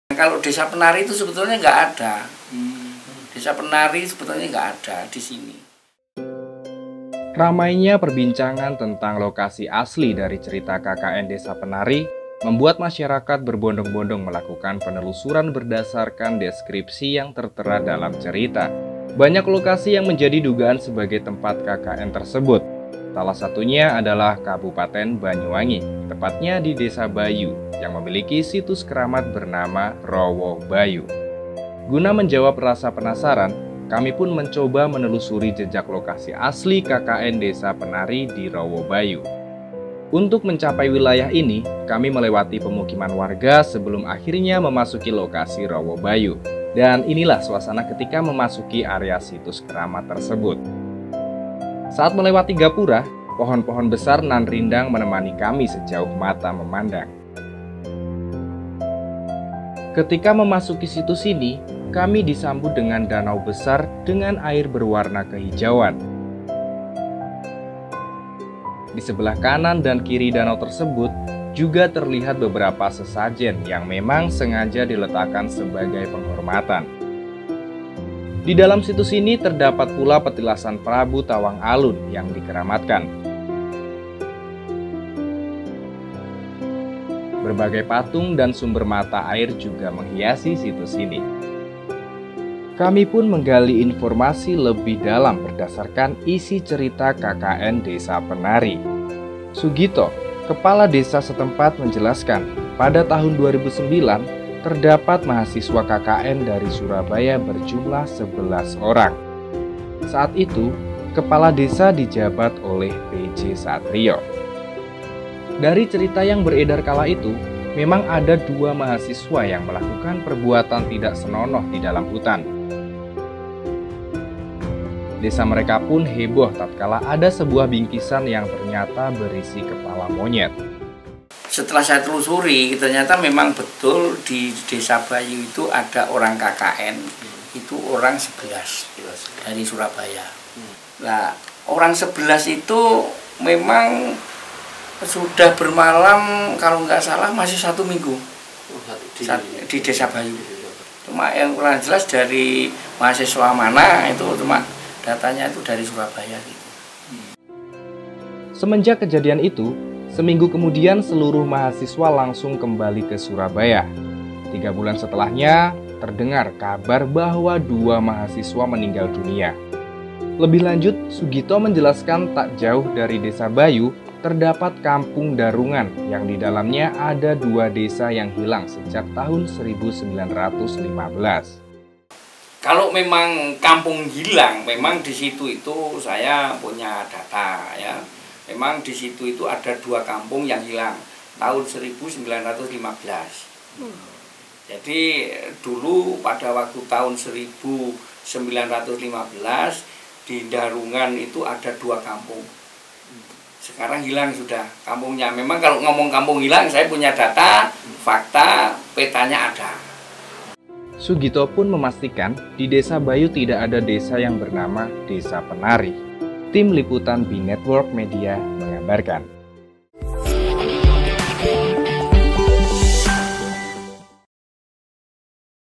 MENELISIK DESA YANG DI SEBUT SEBUT SEBAGAI LOKASI KKN DESA PENARI
BINETNEWS, Ramainya Perbincangan Tentang Lokasi Asli Dari Cerita Kkn Desa Penari Membuat Masyarakat Berbondong-Bondong Melakukan Penelusuran Berdasarkan Deskripsi Yang Tertera Dalam Cerita.

Banyak Lokasi Yang Menjadi Dugaan Sebagai Tempat Kkn Tersebut Salah Satunya Adalah Kabupaten Banyuwangi Tepatnya Di Desa Bayu Yang Memiliki Situs Keramat Bernama Rowo Bayu. Guna Menjawab Rasa Penasaran Kami Pun Mencoba Menelusuri Jejak Lokasi Asli Kkn Desa Penari Di Rowo Bayu.

Untuk Mencapai Wilayah Ini Kami Melewati Pemukiman Warga Sebelum Akhirnya Memasuki Lokasi Rowo Bayu. Dan Inilah Suasana Ketika Memasuki Area Situs Keramat Tersebut. Saat Melewati Gapura  Pohon-Pohon Besar Nan Rindang Menemani Kami Sejauh Mata Memandang.
Ketika Memasuki Situs Ini Kami Disambut Dengan Danau Besar Dengan Air Berwarna Kehijauan. Disebelah Kanan Dan Kiri Danau Tersebut Juga Terlihat Beberapa Sesajen Yang Memang Sengaja Diletakkan Sebagai Penghormatan. Didalam Situs Ini Terdapat Pula Petilasan Prabu Tawang Alun Yang Dikeramatkan. Berbagai Patung Dan Sumber Mata Air Juga Menghiasi Situs Ini.

Kami Pun Menggali Informasi Lebih Dalam Berdasarkan Isi Cerita Kkn Desa Penari. Sugito Kepala Desa Setempat Menjelaskan Pada Tahun 2009 Terdapat Mahasiswa Kkn Dari Surabaya Berjumlah 11 Orang. Saat Itu Kepala Desa Di Jabat Oleh P.J Satrio.

Dari Cerita Yang Beredar Kala Itu Memang Ada Dua Mahasiswa Yang Melakukan Perbuatan Tidak Senonoh Didalam Hutan Desa Mereka Pun Heboh Tatkala Ada Sebuah Bingkisan Yang Ternyata Berisi Kepala Monyet.